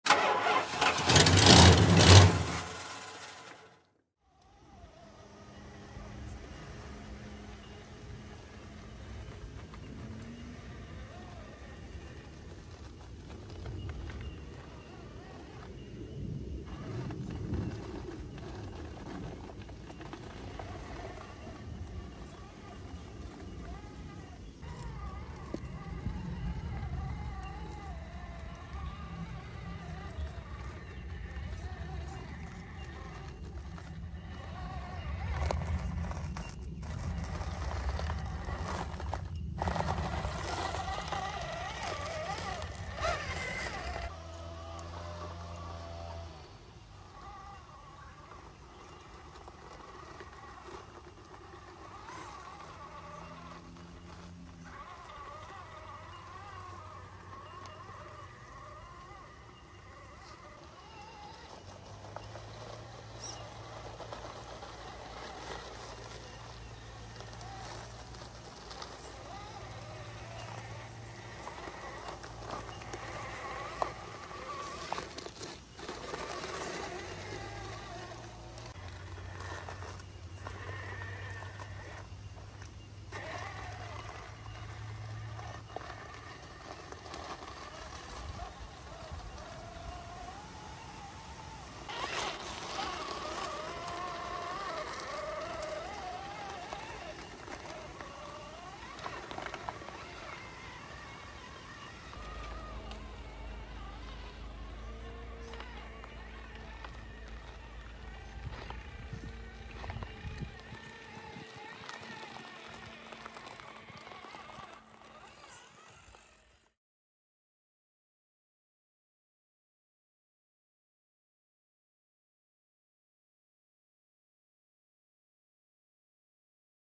Scale Driven: 1:10 Scale RC4WD Chevy K5 Blazer controlled ascent | ASMR 🎧📺
Welcome back to the channel RC friends!

Here’s my K5 Blazer on a scale climb at my local trail.

Tires are Ali Express clone BFGs on Gmade pastic beadlock wheels, front weighted.

Scale RC4WD scale hubs.

Single leaf springs on all corners (medium and short helpers removed) No chino mod. Internal springs removed.

Hobbywing WP1080 ESC
Stock 35T motor

Custom weathering using sandpaper, filing tools and Tamiya weathering set

Cheers,
Jono